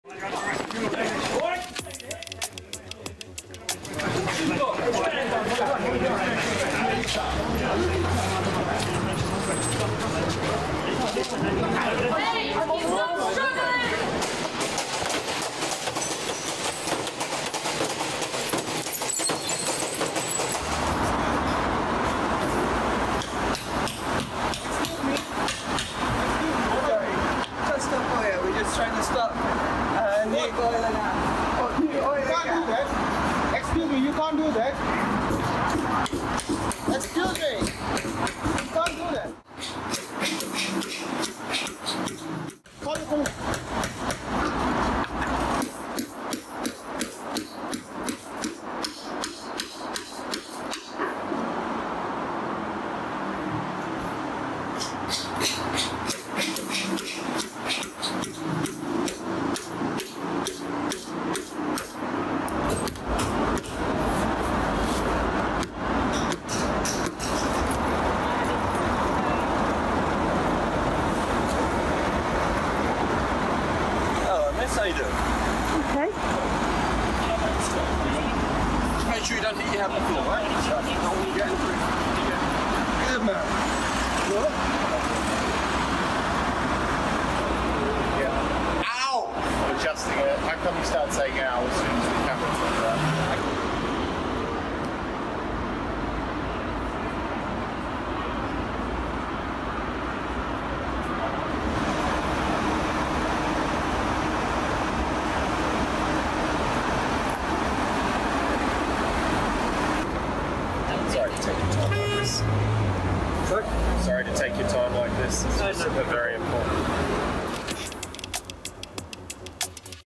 Climate activists attack petrol pumps in London
Just Stop Oil activists have damaged petrol pumps in London to protest against the issuing of new oil and gas licences in the UK.